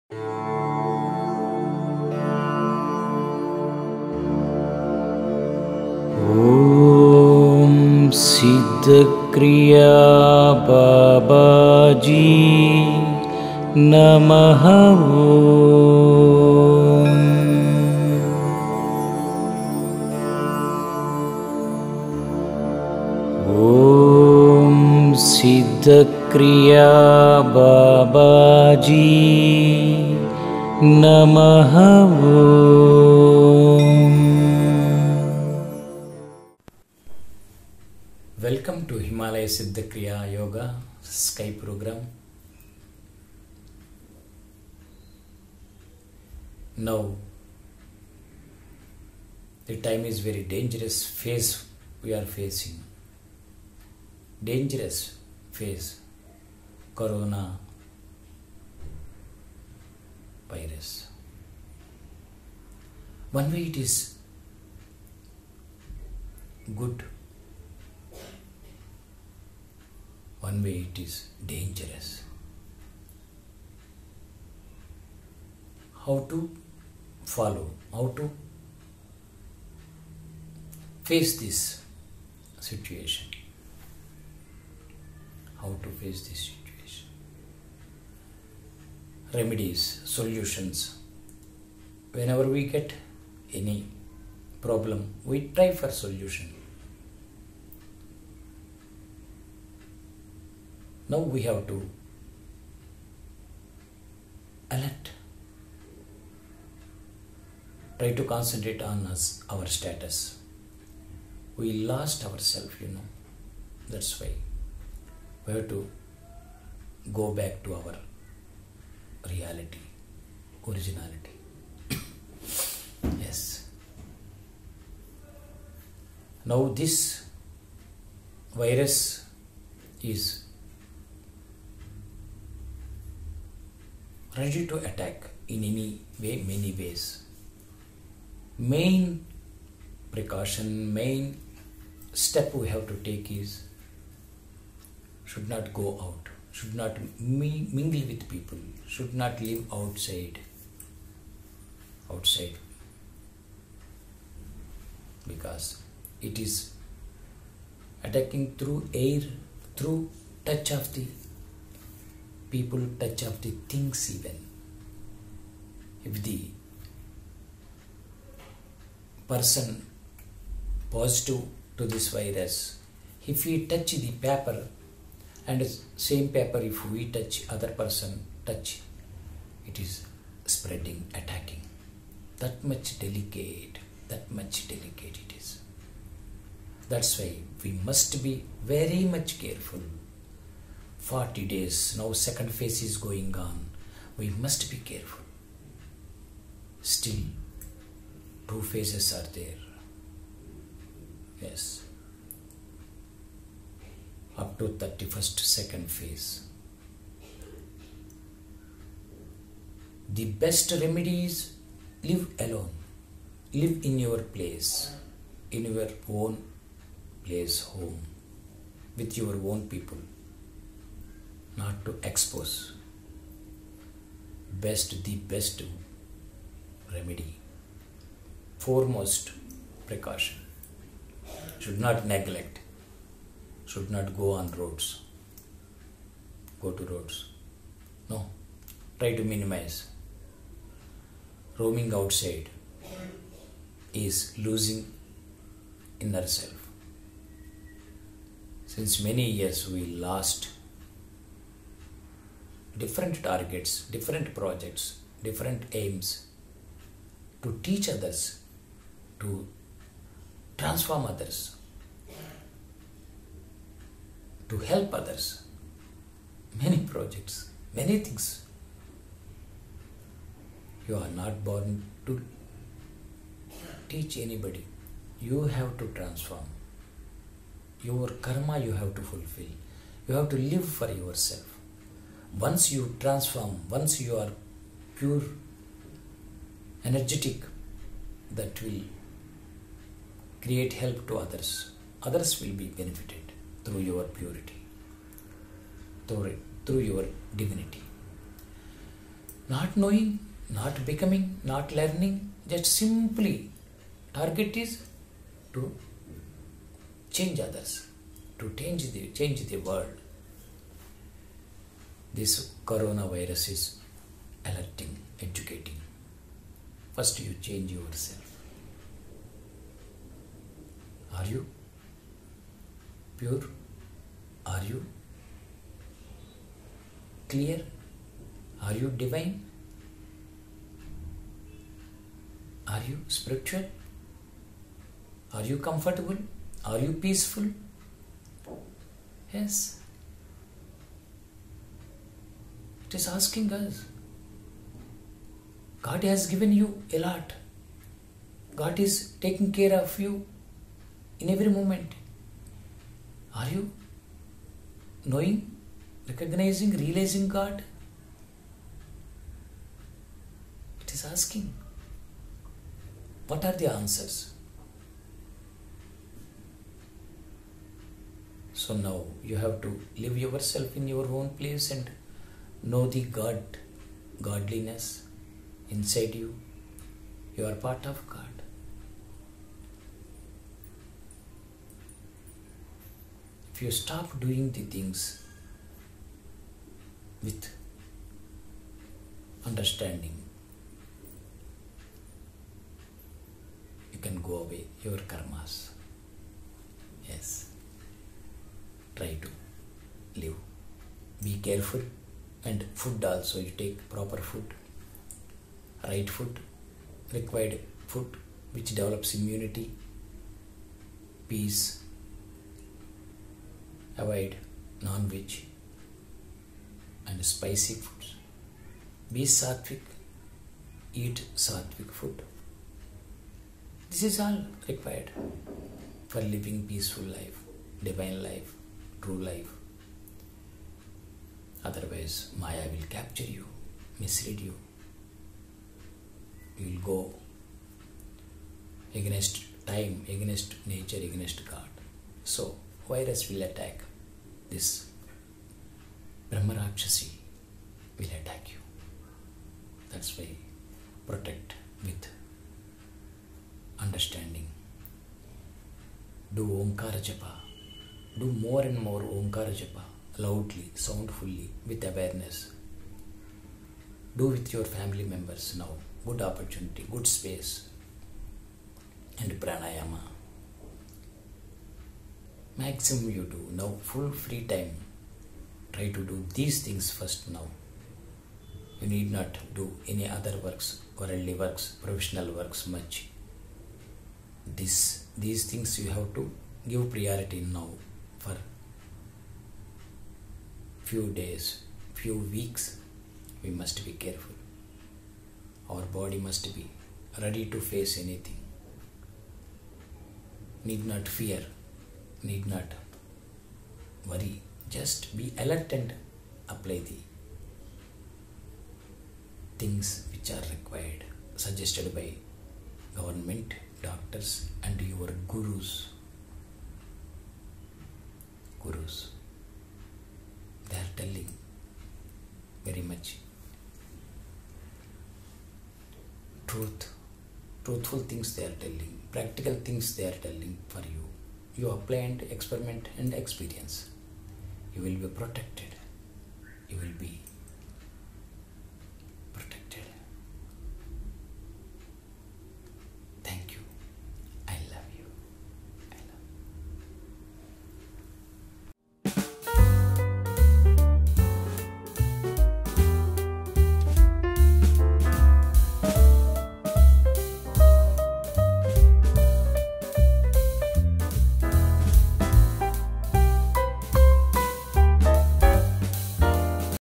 CORONA - Don’t take it Lightly - Very DANGEROUS
VEDA FOUNDATION# SKY# Himalaya#
ATTENTION...REALISATION
We are living in Dangerous period.
 Never before.
 Time to be alert and try to transform. Become Human.
 Realize Three Qualities... Right food - Pure thoughts -Perfect Actions.
Thousands of people dying.
 Be serious. Live in ISOLATION - Live in House - Be DIVINE.
.Avatar Sri Vedananda Babaji